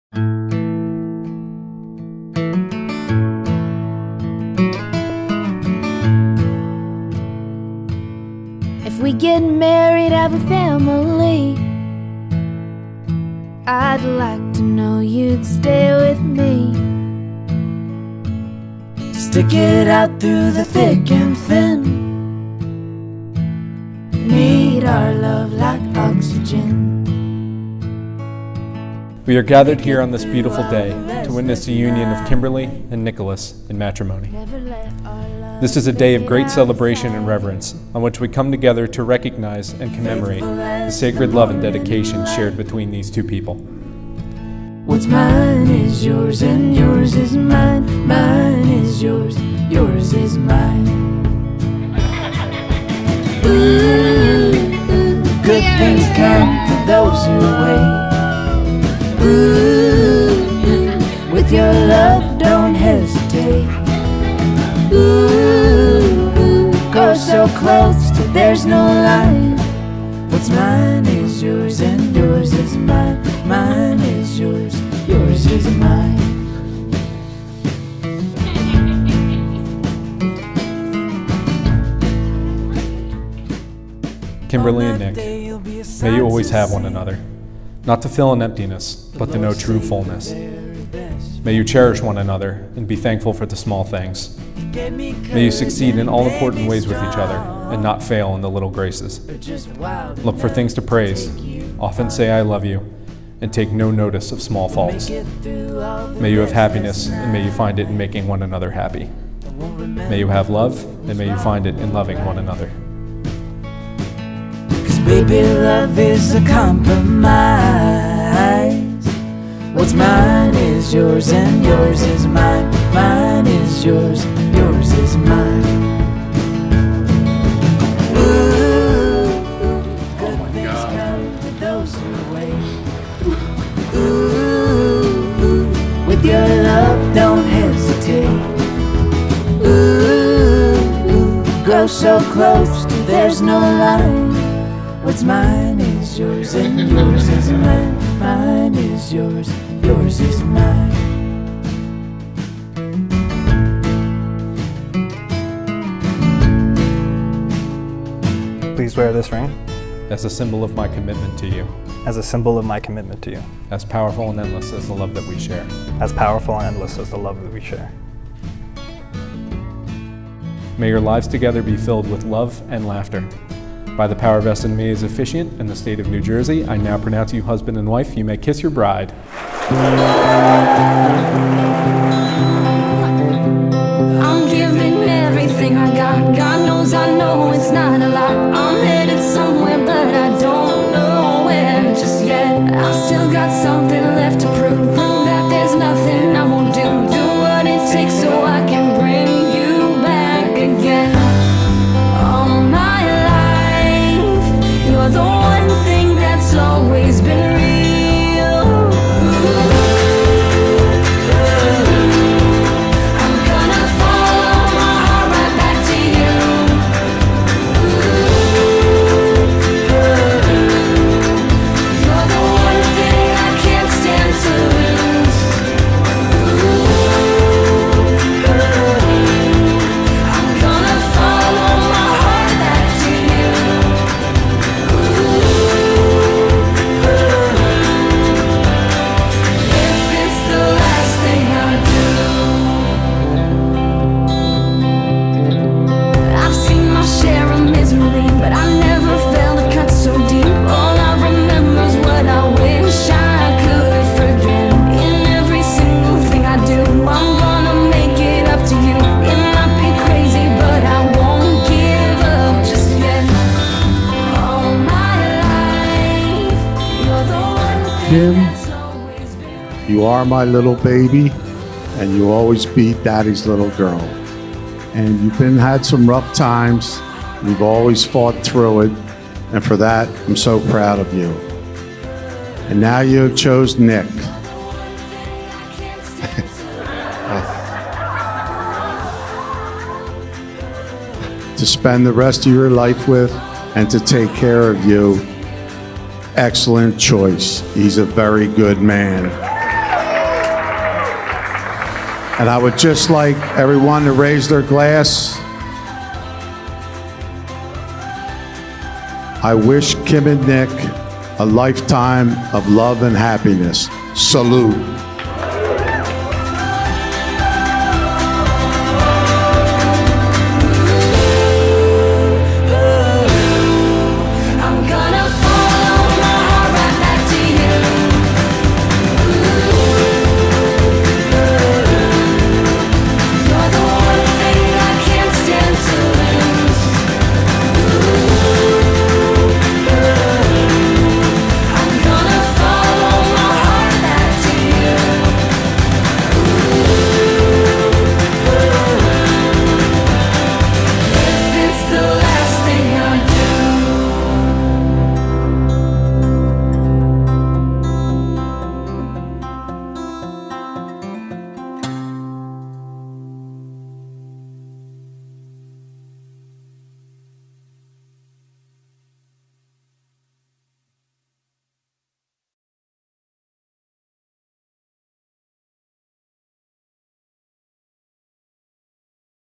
Chapel Wedding at Brigalias {Kimberly & Nicholas} NJ Wedding Video
Kim and Nick celebrated their wedding at Brigalias and Shaun Reilly Photography was with them to capture the special moments as they unfolded. After Kim was ready in her gown from The Curvy Bride and Nick set to go in his tux from Men's Wearhouse, First Class Limousine whisked them off to say "I do." Zenplicity Florist created Kim's bouquet and decorated Brigalia's chapel for the ceremony - and decked out the tables for their exciting reception! Impulse Entertainment DJs started the party with a bang and a spray of sparkles, then Kim, Nick, and all their loved ones danced the night away and sent the new husband and wife into a happy future together.